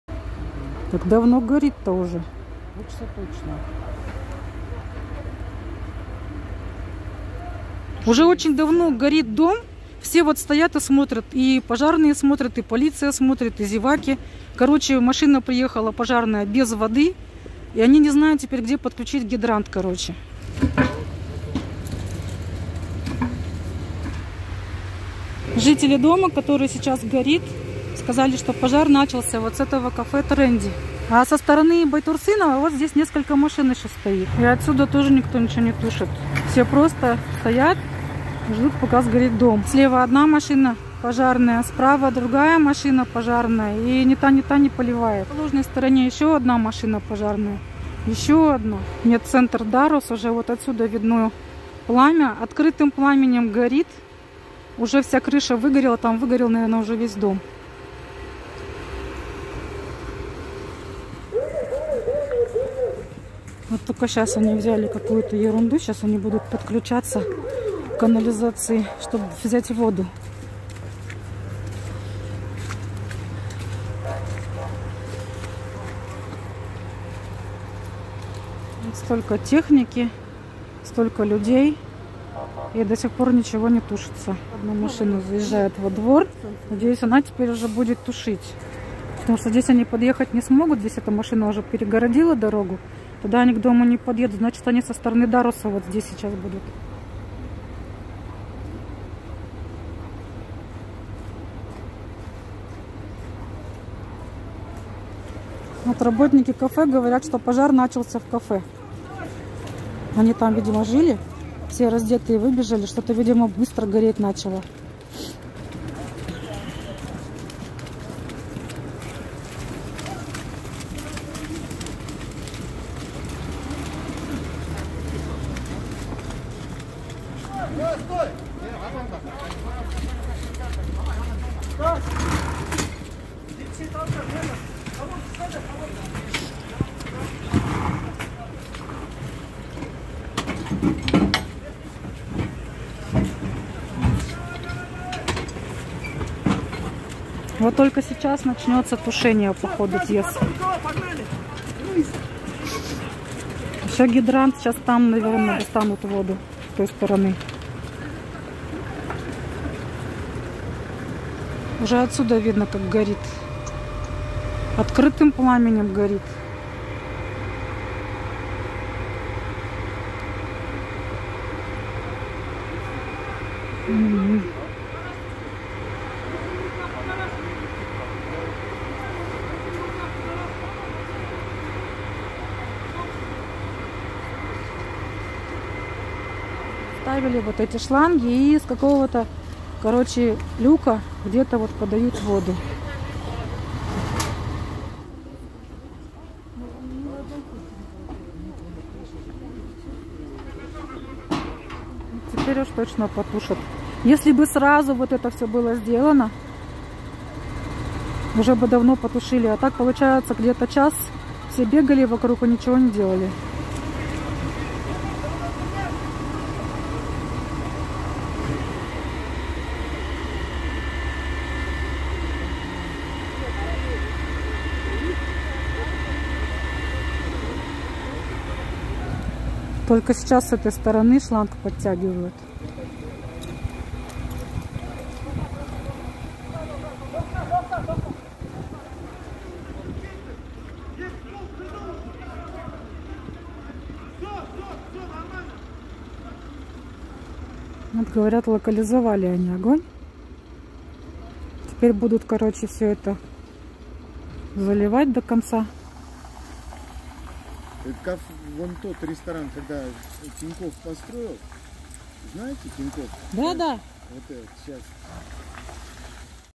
Пожар в Алматы сегодня
Бостандыкский район Байтурсынова между Сатпаева и Абая.